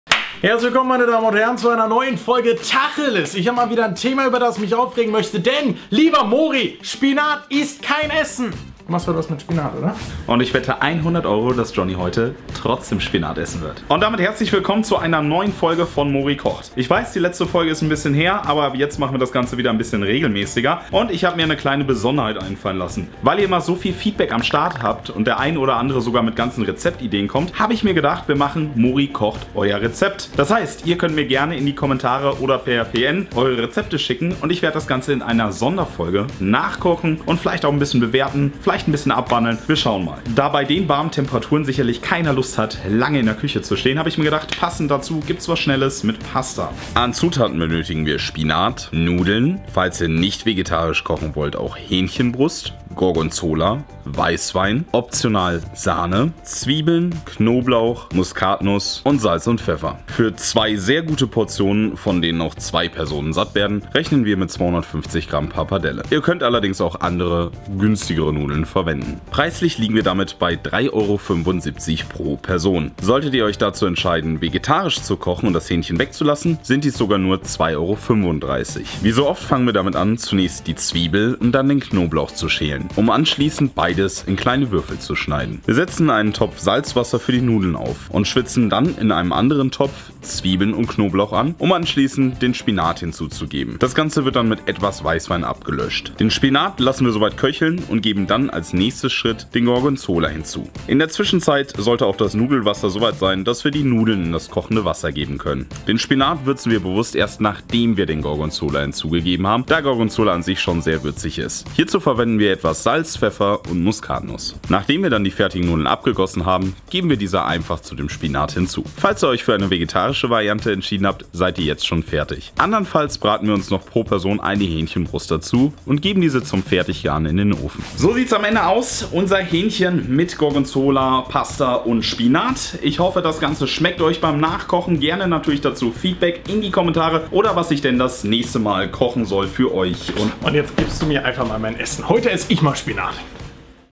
Spinat-Gorgonzola Pappardelle mIt Hähnchenbrust oder vegetarisch | Mori kocht
Mori kocht Spinat-Gorgonzola Pappardelle wahlweise vegetarisch oder mIt Hähnchenbrust. Für die nächste Folge teil mir doch gerne eure Wünsche in den Kommentaren mit! Gerne auch euer Rezept für ein kleines Spezial.

Kleiner Tipp: Falls ihr jungen Spinat verwendet spart ihr euch das entfernen der Stengel (es sei denn ihr wollt es wirklich butterweich haben). Falls ihr wissen wollt welchen Wein ihr zu dem Essen verwendet sollt: Nehmt wie zum kochen auch einen trockenen Weißwein. Ob nun mit Fleisch oder ohne: Lecker ist das Spinat-Gorgonzola Rezept sowieso! Auch an Pasta könnt ihr euch austoben: Pappardelle, Tagliatelle oder Spaghetti. Erlaubt ist was schmeckt. Ideal für den Sommer!

Rezept (für 2 Personen):

400g Hähnchenbrust (optional)
300g Junger Spinat
250g Pappardelle
200g Gorgonzola
0,15L Weißwein
1 Zwiebel
1 Knoblauchzehe
Salz, Pfeffer und Muskatnuss

Zunächst die Zwiebel und den Knoblauch schälen und in kleine Würfel schneiden. Diese dann in etwas Öl anschwitzen und den jungen Spinat hinzugeben. Falls ihr es weicher mögt, müsst ihr die Stengel entfernen. Dies ist besonders bei Blattspinat zu empfehlen. Auf jeden Fall den Salat vorher waschen. Solltet ihr das Gericht nicht gleich essen unbedingt vorher Blanchieren um der Nitrit-Bildung vorzubeugen.

Den Spinat mit Weißwein ablöschen und etwas köcheln lassen. Anschließend den Gorgonzola hinzugeben. Erst dann mit Muskat, Salz und Pfeffer würzen, da Gorgonzola schon sich sehr würzig ist. Anschließend die gekochte Pappardelle beigeben.

Wahlweise könnt ihr parallel noch Hähnchenbrust braten und hinzugeben.

► Meine Tastatur*
► Mein Headset*

Spezielle Gäste: LPG Just Johnny